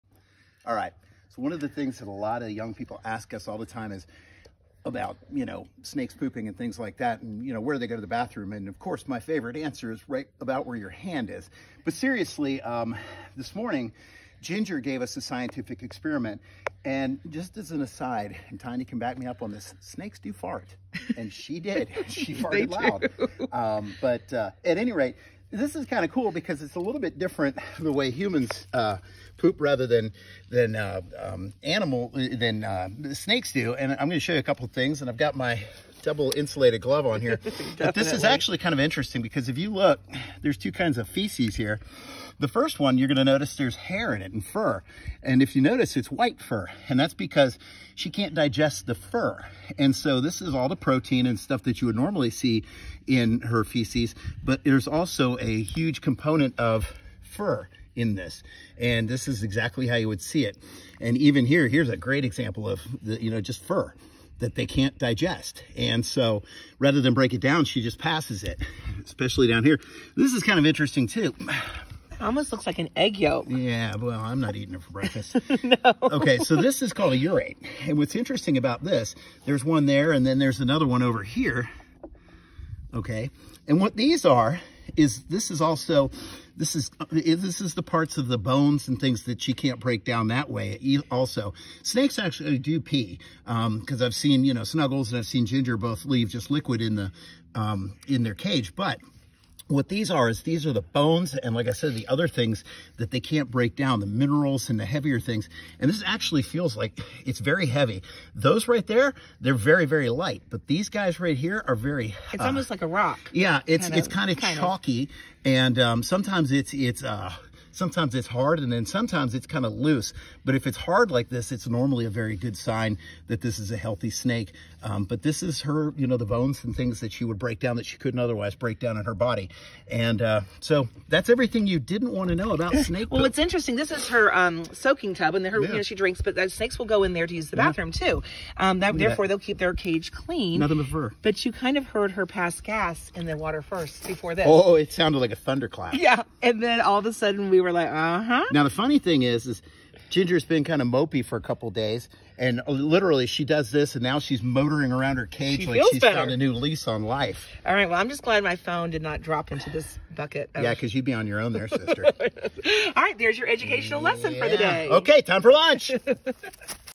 What’s in a snake’s poop?
What’s in a snake’s poop? Ginger, our Burmese Python, decided to help answer that question by providing us a sample for you to see up close! Students (and teachers) frequently ask us about how snakes poop and what it looks like, so here it is! 💩